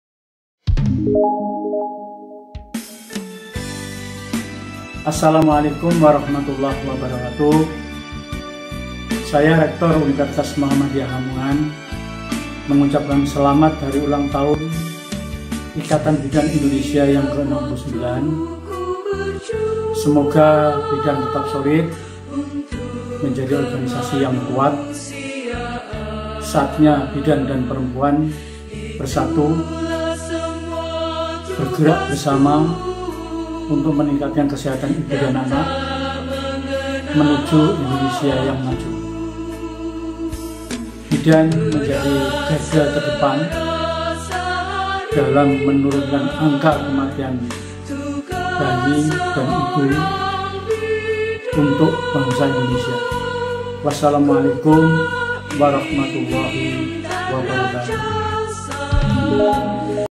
SELAMAT HUT IBI KE 69 | REKTOR UM LAMONGAN
Selamat Hari Bidan Nasional para srikandi Ibu Pertiwi. Semoga Profesi Bidan menjadi lebih baik dalam melayani dan memberi arti pada seluruh keluarga NKRI. Kelahiran dan perjuangan adalah dua hal yang tidak dapat dipisahkan. Begitu pun antara kehidupan dan Bidan. Tak ada yang lebih mulia daripada sebuah pekerjaan yang memperjuangkan selembar nyawa kehidupan. Itulah Bidan. Dan kami bangga menjadi salah satu bagian dari profesi yang penuh akan pertanggungjawaban. Selamat Hari Bidan Nasional! Jayalah seluruh Bidan Indonesia.